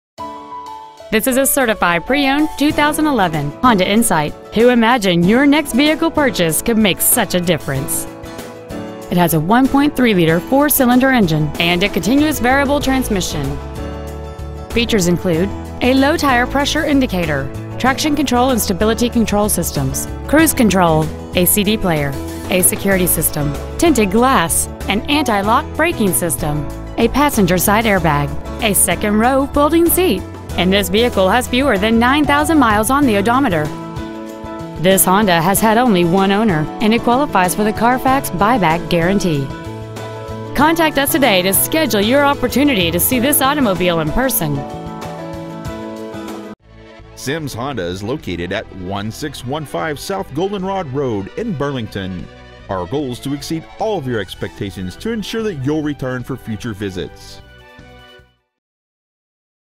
2011 Honda Insight Certified Burlington WA 98226
We are proud to present this 2011 Honda Insight .

 Year : 2011
 Make : Honda
 Model : Insight
 Engine : 1.3L I4 SOHC 8V HYBRID
 Trans . : CVT
 Exterior : Silver
 Miles : 8,960
 Interior : Gray
 Sims Honda
 1615 S. Goldenrod Road
 Burlington , WA 98233
 2011 Honda Insight Burlington WA